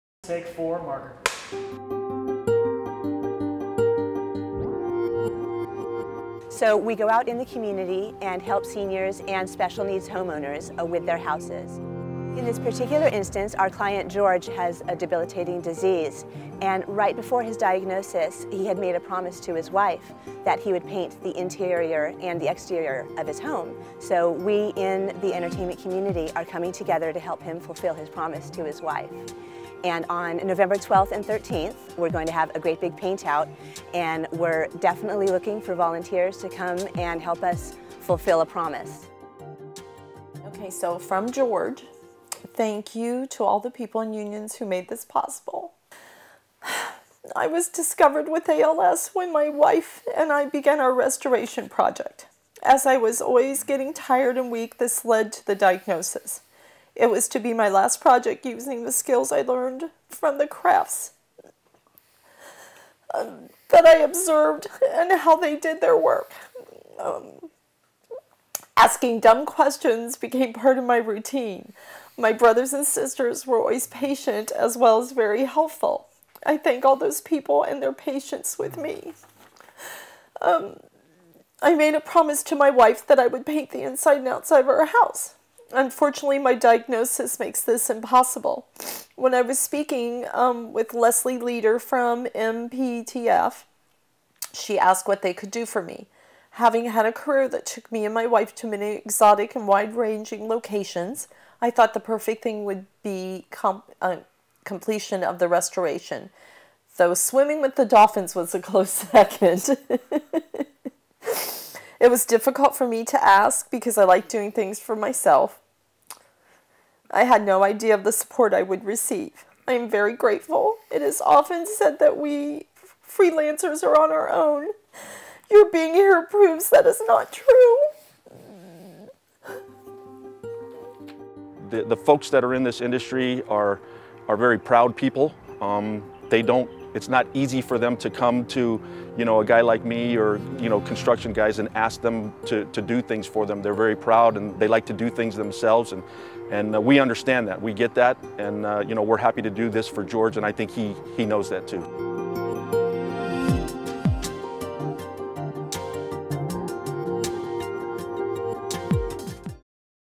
Take Action: Keeping A Promise
On Nov. 12th & 13th, 2011 the entertainment industry is coming together to help one of our own fulfill a final promise to his wife, and we need your help. Longtime location manager and Local 399 member George Herthel made his wife a simple promise: he would repaint the interior and exterior of their home. Things became much more complicated, however, when George was diagnosed with ALS, a debilitating disease that has taken away his ability to walk and speak. Watch this video to hear George's moving story in his own words. For more information on other volunteer opportunities at MPTF